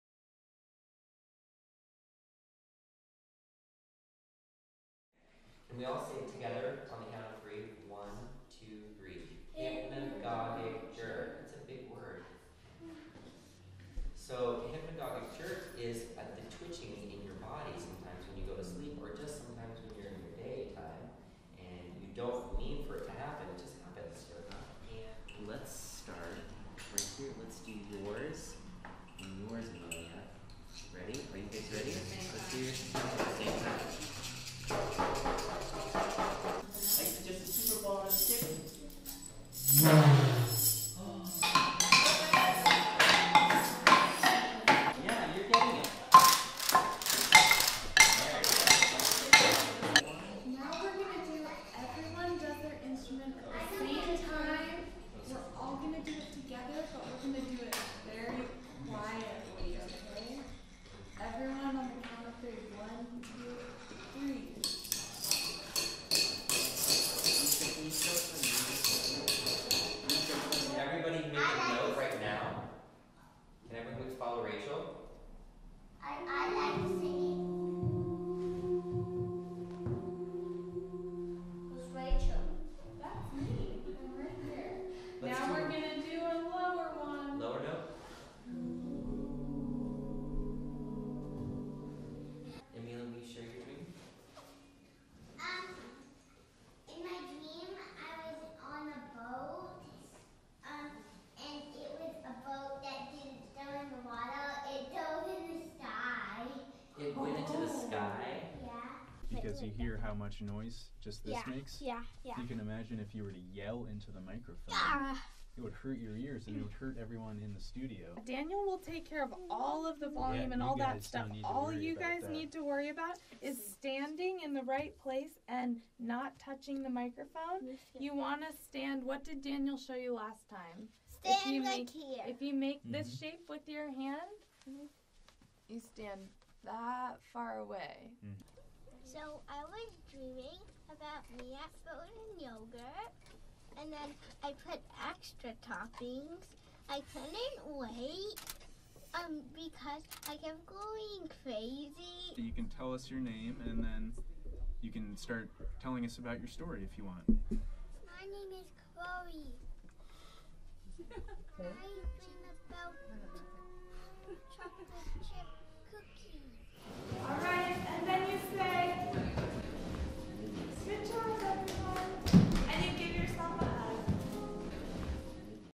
Jack Straw/AVIA Gallery Workshops Spring 2014
In April 2014 Jack Straw Cultural Center and Arts and Visually Impaired Audiences (AVIA) presented a series of accessible workshops conjunction with Rachel Green and Seth Sexton's New Media Gallery installation Hypnagogic Jerk, which incorporates dance, sculpture, and sound. Over three consecutive weekends, sighted and visually impaired students from Seattle and around the region participated in hands-on art, music, and movement workshops with Seth, Rachel, and Jack Straw teaching artists Susie Kozawa and Esther Sugai. This project was provided under a contract with the John F. Kennedy Center for the Performing Arts as part of their VSA Arts Access for Kids program. Video edited by Alexander Karbo.